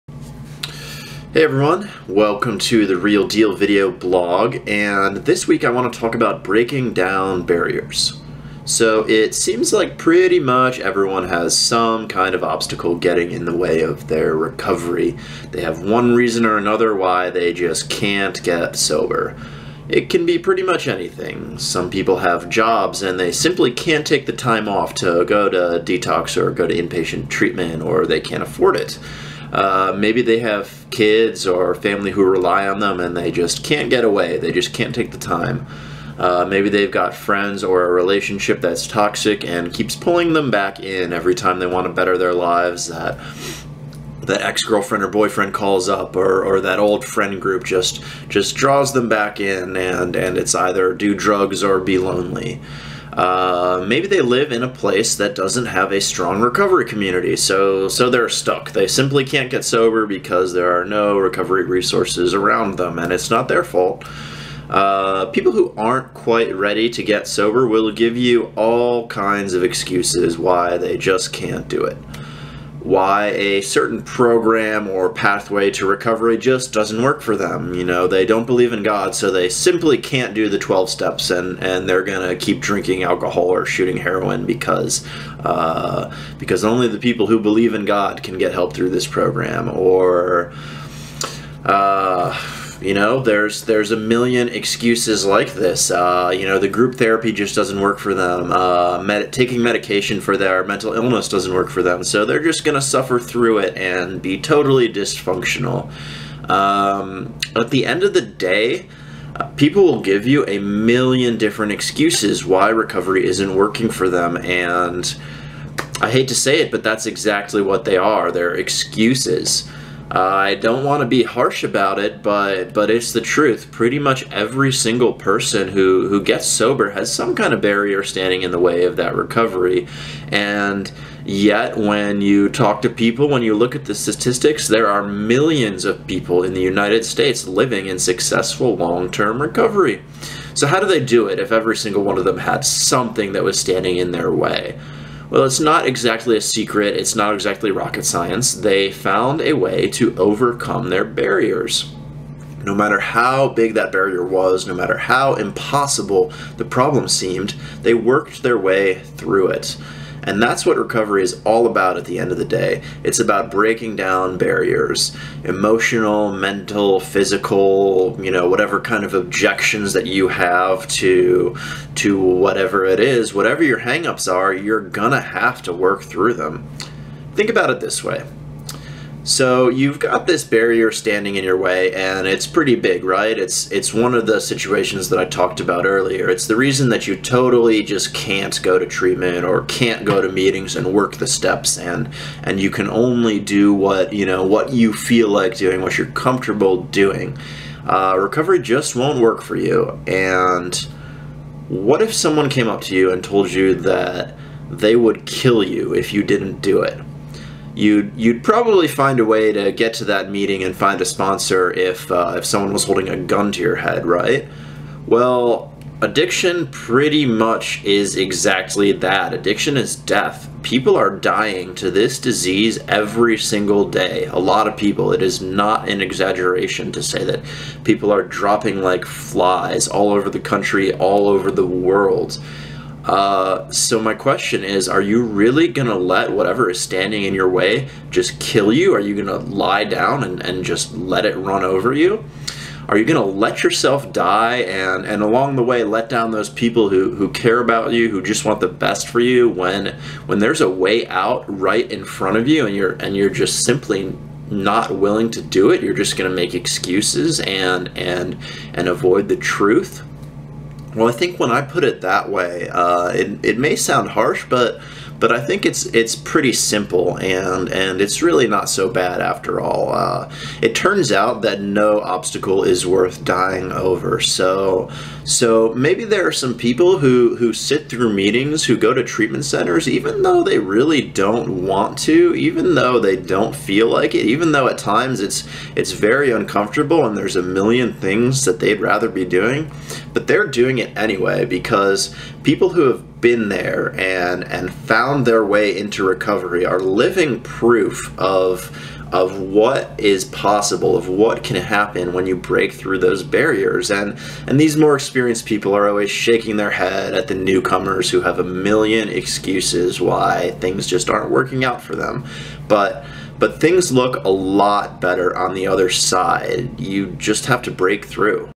Breaking Down Barriers
Everyone has some kind of barrier or excuse in the way of finding recovery. If they want to succeed, they'll need to break through.